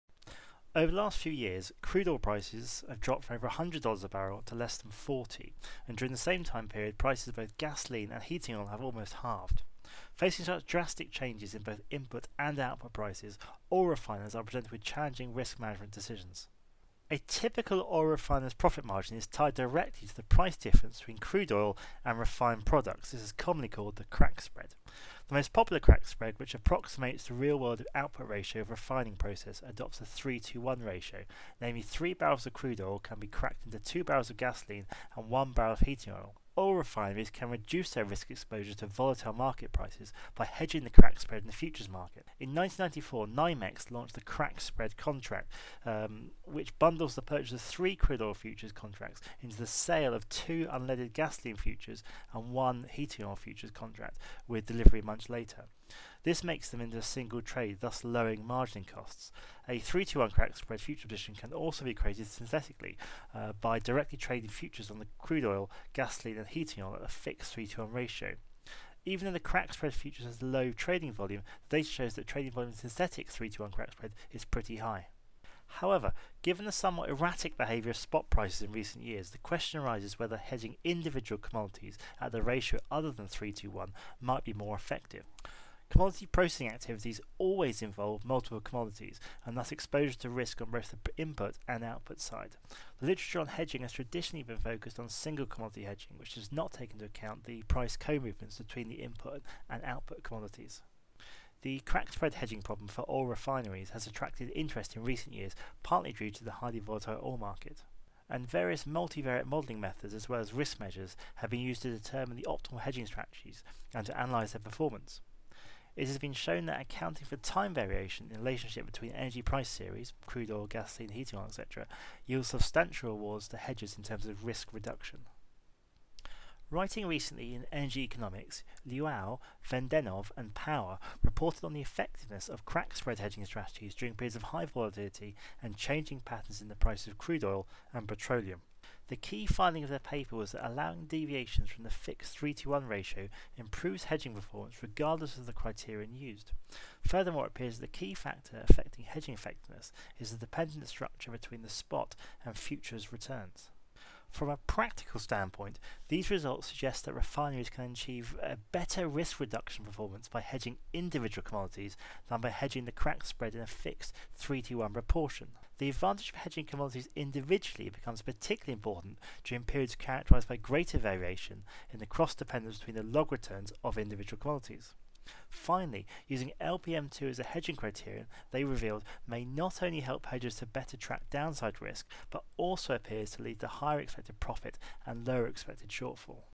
Optimal Crack Spread Hedging
The traditional approach to hedging the crude oil refining margin (crack spread) adopts a fixed 3:2:1 ratio between the futures positions of crude oil, gasoline, and heating oil. The latest research indicates that this might not be the optimal approach